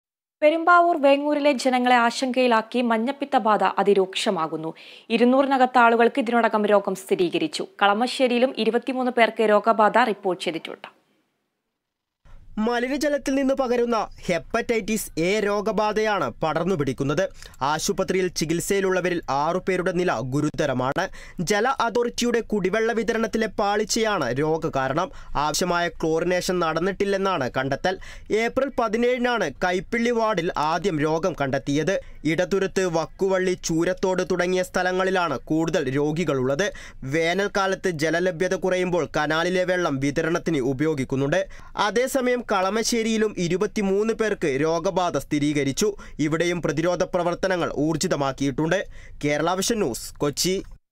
വേങ്ങൂരിൽ ജനങ്ങളെ ആശങ്കയിലാക്കി മഞ്ഞപ്പിത്ത ബാധ അതിരൂക്ഷമാകുന്നു | Ernakulam
പെരുമ്പാവൂര്‍ വേങ്ങൂരിലെ ജനങ്ങളെ ആശങ്കയിലാക്കി മഞ്ഞപ്പിത്ത ബാധ അതിരൂക്ഷമാകുന്നു. 200നടുത്ത് ആളുകള്‍ക്ക് ഇതിനോടകം രോഗം സ്ഥിതീകരിച്ചു. കളമശ്ശേരിയിലും 23 പേര്‍ക്ക് രോഗബാധ റിപ്പോര്‍ട്ട് ചെയ്തിട്ടുണ്ട്.

About COA